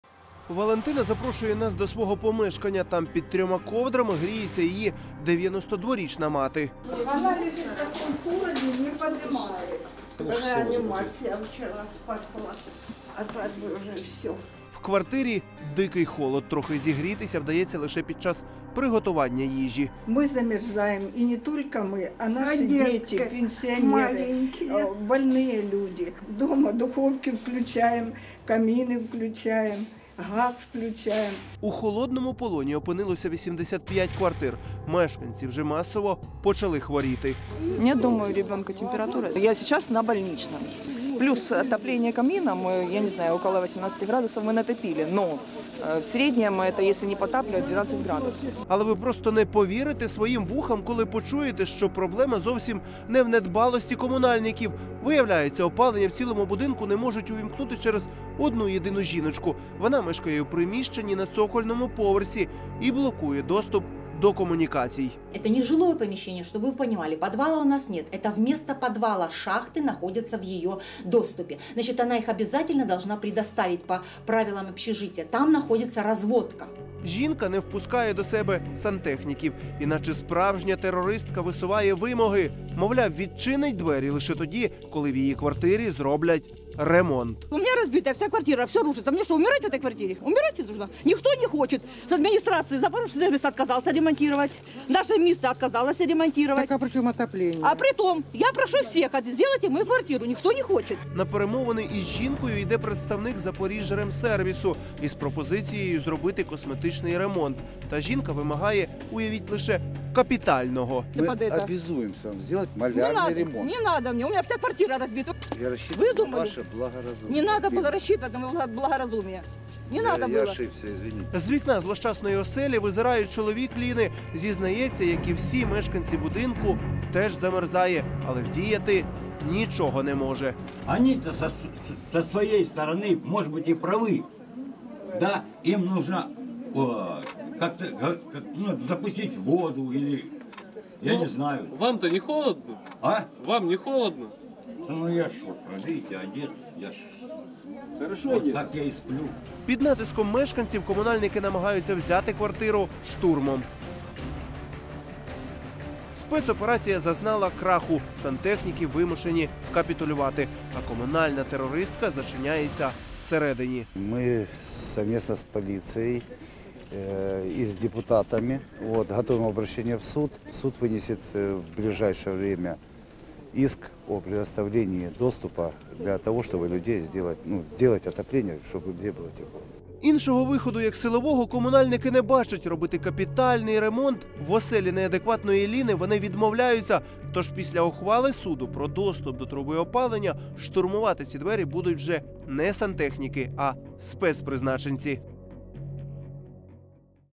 У Запоріжжі через "комунальну терористку" 80 квартир залишилися без опалення
Жінка запрошує до пемешкання. Там під 3-мя ковдрами лежить її 90-річна мати. У холодному полоні опинилися 80 квартир. Виявляється опалення не можуть увімкнути через одну єдину жінку. Вона не пускає сантехніків та вимагає ремонту від комунальників.